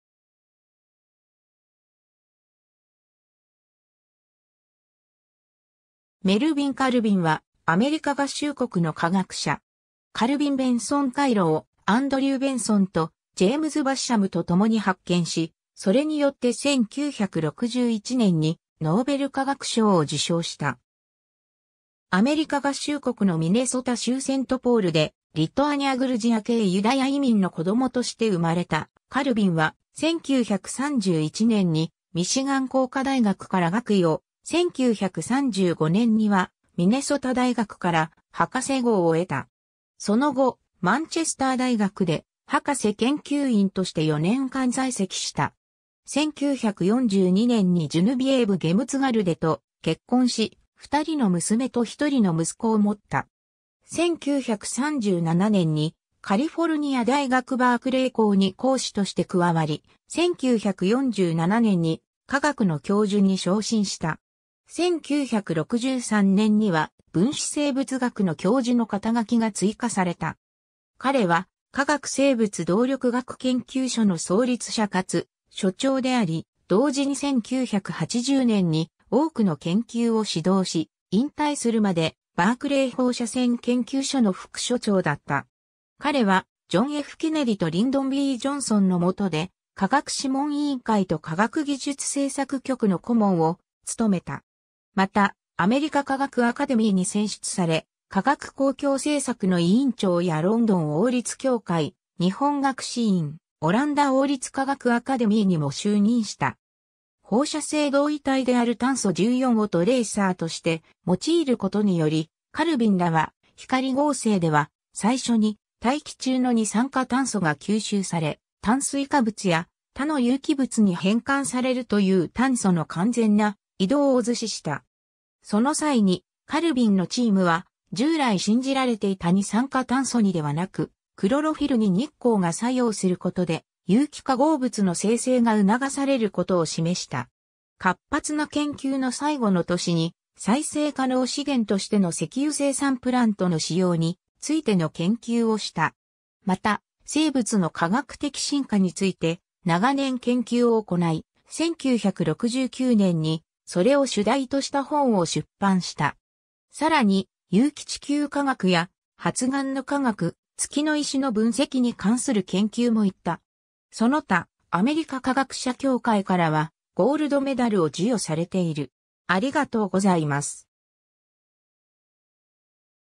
メルヴィン・カルヴィン
ウィリアム・H・ニコルズ賞の受賞者
ローレンス・バークレー国立研究所の人物
メルヴィン・カルヴィン（Melvin Calvin, 1911年4月8日 – 1997年1月8日）はアメリカ合衆国の化学者。
カルビン・ベンソン回路をアンドリュー・ベンソンとジェームズ・バッシャムと共に発見し、それによって1961年にノーベル化学賞を受賞した。
 アメリカ合衆国のミネソタ州セントポールでリトアニア・グルジア系ユダヤ移民の子供として生まれたカルヴィンは、1931年にミシガン工科大学から学位を、1935年にはミネソタ大学から博士号を得た。
その後、マンチェスター大学で博士研究員として4年間在籍した。
1942年に Genevieve Jemtegaard と結婚し、2人の娘と1人の息子を持った。
 1937年にカリフォルニア大学バークレー校に講師として加わり、1947年に化学の教授に昇進した。
1963年には分子生物学の教授の肩書きが追加された。
彼は化学生物動力学研究所の創立者かつ所長であり、同時に1980年に多くの研究を指導し、引退するまでバークレー放射線研究所の副所長だった。
 彼はジョン・F・ケネディとリンドン・B・ジョンソンのもとで科学諮問委員会と科学技術政策局の顧問を勤めた。
また、アメリカ科学アカデミーに選出され、科学公共政策の委員長やロンドン王立協会、日本学士院、オランダ王立科学アカデミーにも就任した。
 放射性同位体である炭素14をトレーサーとして用いることにより、カルヴィンらは、光合成では最初に大気中の二酸化炭素が吸収され、炭水化物や他の有機物に変換されるという炭素の完全な移動を図示した。
その際にカルヴィンのチームは従来信じられていた二酸化炭素にではなく、クロロフィルに日光が作用することで有機化合物の生成が促されることを示した。
活発な研究の最後の年に再生可能資源としての石油生産プラントの使用についての研究をした。
また、生物の化学的進化について長年研究を行い、1969年にそれを主題とした本を出版した。
さらに、有機地球化学や発癌の化学、月の石の分析に関する研究も行った。
 その他、アメリカ化学者協会 (American Institute of Chemists) からはゴールドメダルを授与されている。